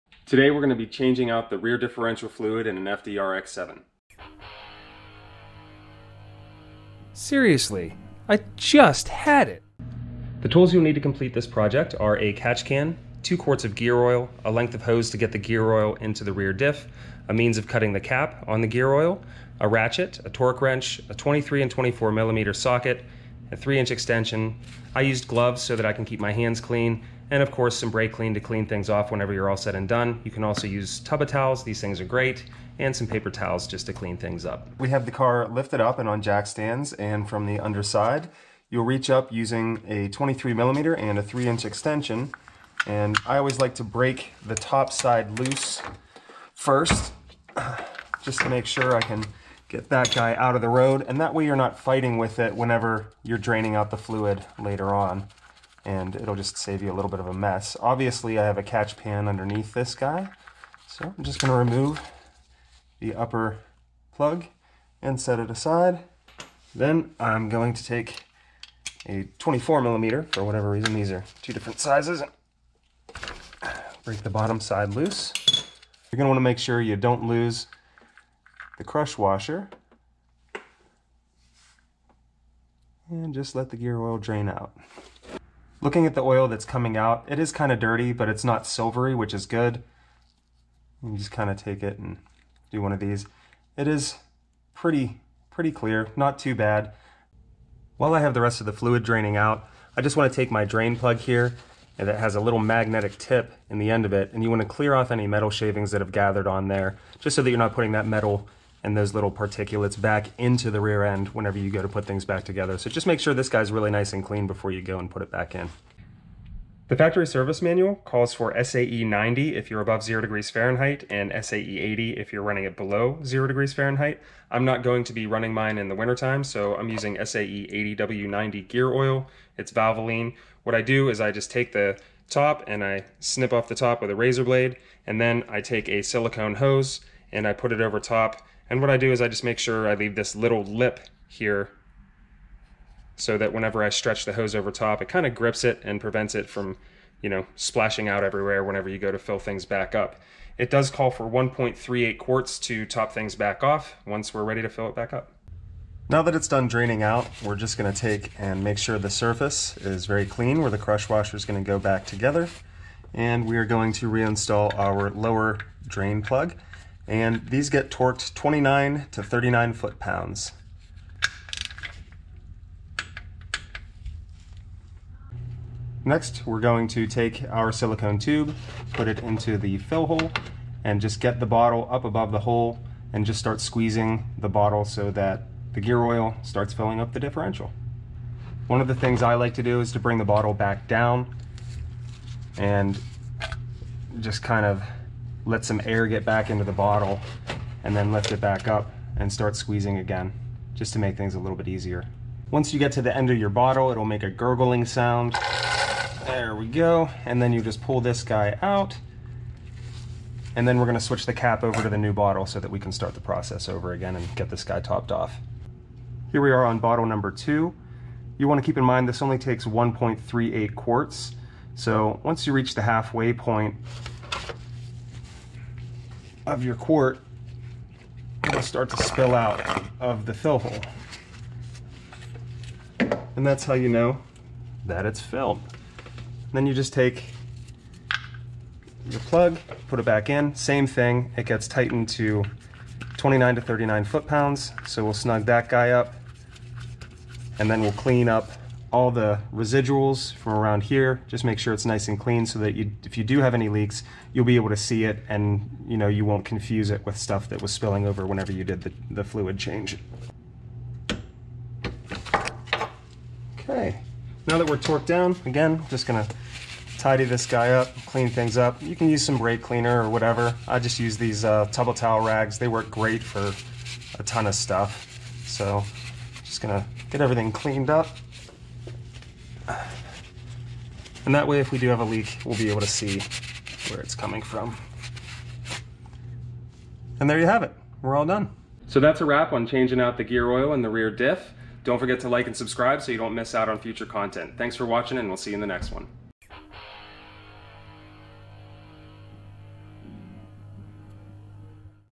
How to change out the rear differential fluid in a 93-02 RX-7.
in this video I show you how to change out the rear differential fluid in a third gen Mazda RX-7.